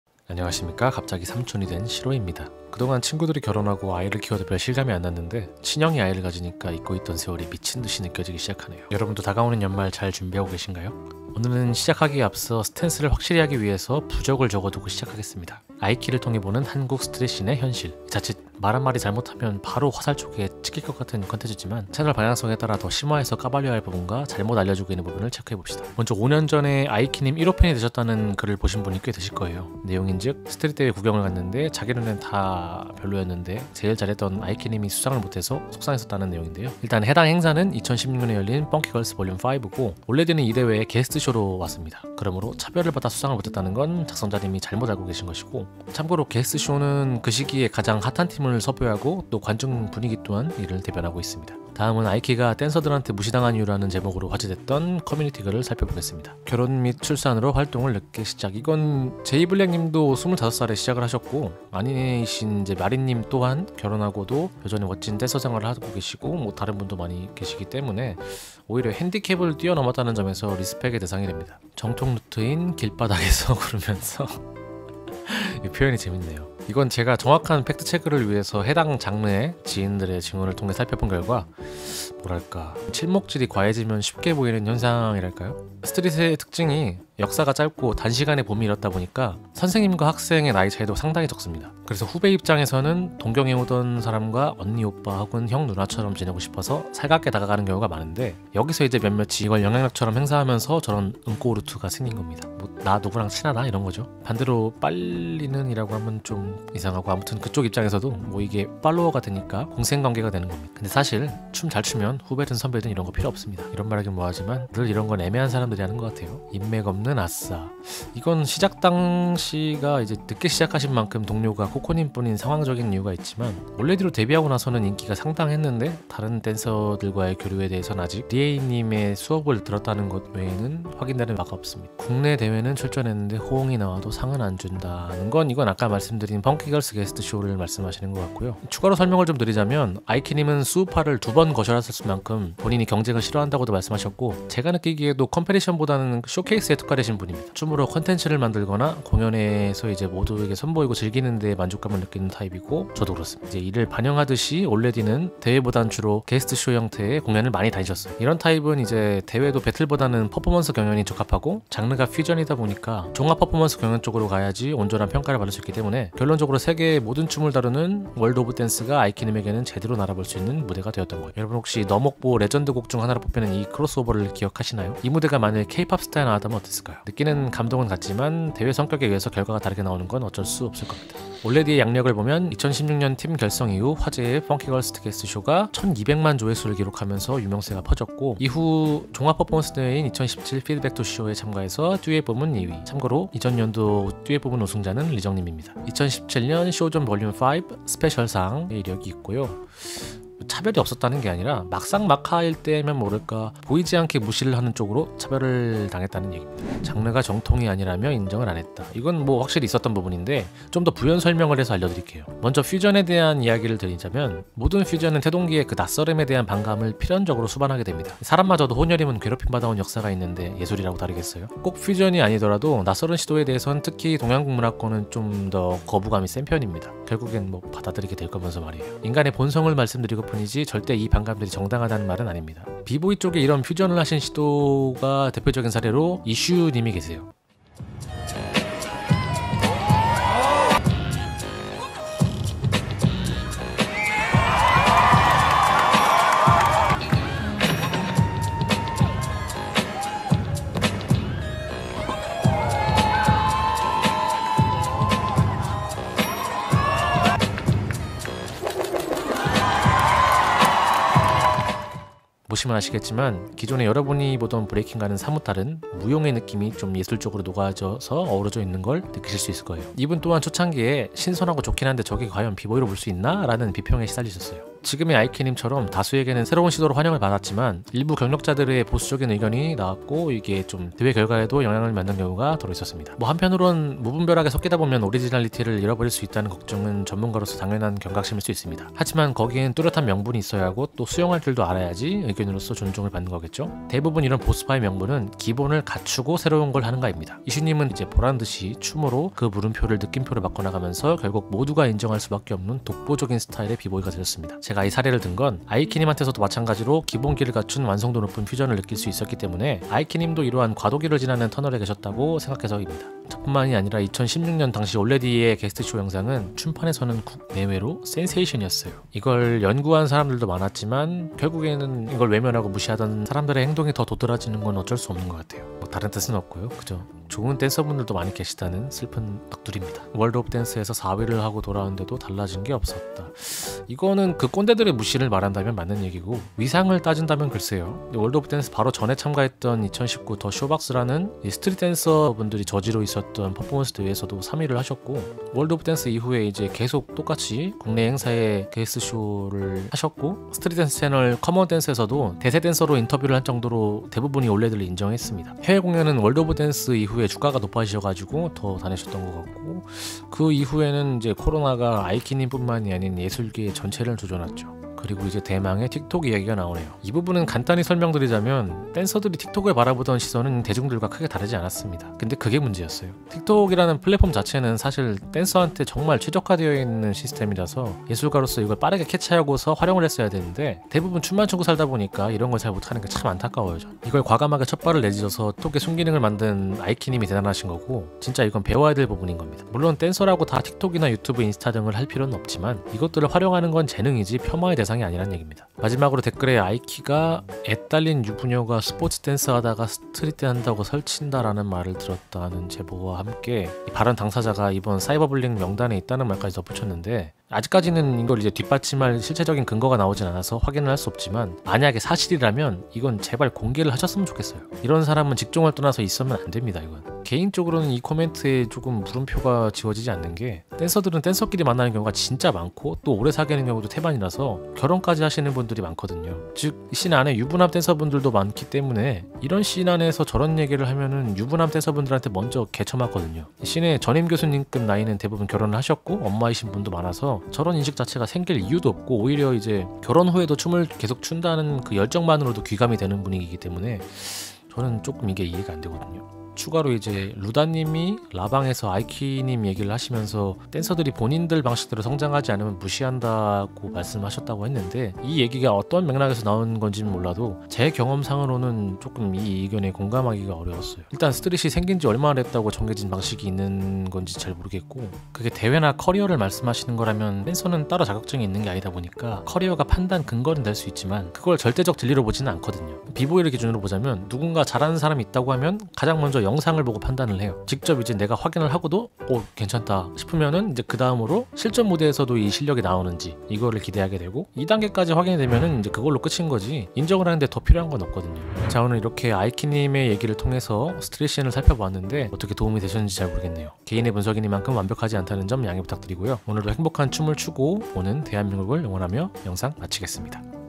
아이키를 통해 보는 한국 스트릿의 현실
0:26 킹랑님의 제보로 5년전 1호팬이 되신분이 보신 대회는 펑키걸스 게스트쇼가 아닌
제 미숙함으로 혼동을 드려 죄송합니다.
블로그상에 사용하신 짤을보고 펑키걸스로 파악했는데
작성자분도 팬이 된 김에 영상을 찾아보시다가 펑키걸스 때와 같은 의상인걸 보고 영상을 쓰신 것 같습니다. 실제론 의상만 같고 전반부가 다른 퍼포먼스로 대회를 참가하셨네요.
소중한 제보 정말 감사드리고 앞으로 영상제작에 더욱 신중을 기하겠습니다. 시청에 불편을드려 죄송합니다. 감사합니다.

춤을 사랑하는 모두가 존중받고 행복했으면 합니다:)